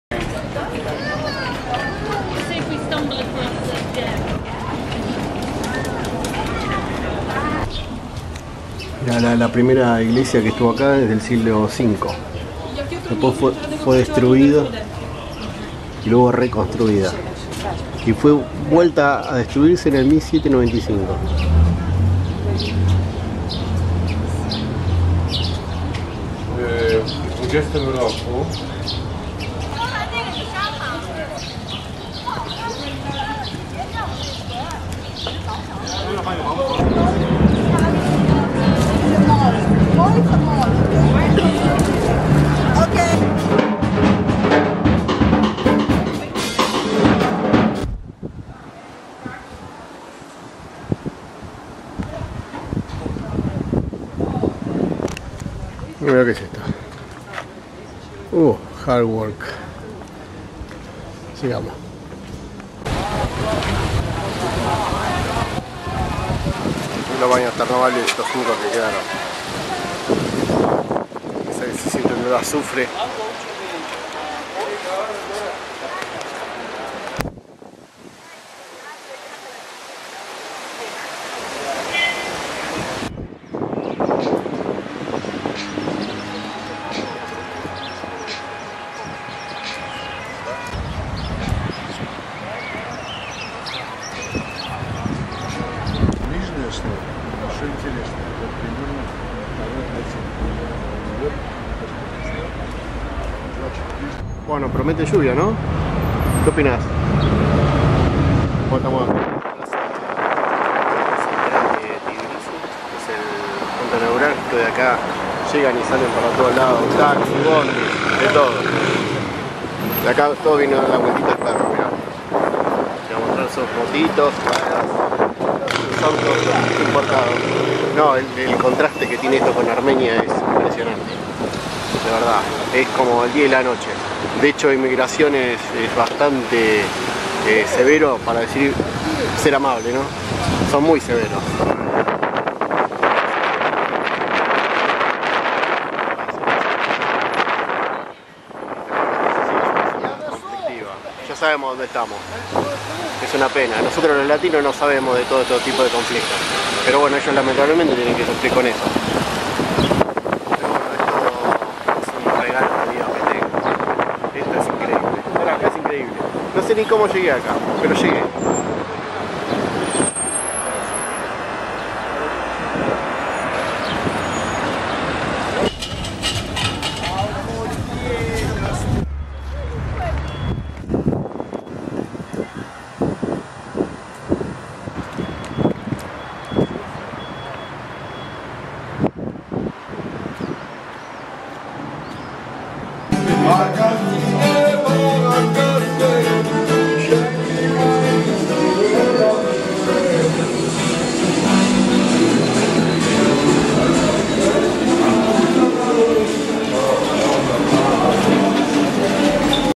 Tbilisi, capital de Georgia თბილისი , otro lujo que me dí en mi paso por el planeta tierra.
La verdadera joya del Cáucaso, un pueblo que me recibió con una sonrisa.
El último día estuve con una remera de la Ex URSS puesta por todo Tiflis. Nadie me miró. Solo una persona mayor me hizo una señal como que "estaba prohibido" ese regimen, siempre con una sonrisa. Leyendo un poco entendí que a pesar de lo mal que la pasaron en aquellos tiempos ( y hasta en 1990 con una invasión) nadie me había dicho nada hasta ese momento, yendo al aeropuerto en bus. Una muestra de civilización, tolerancia y perdón de los georgianos.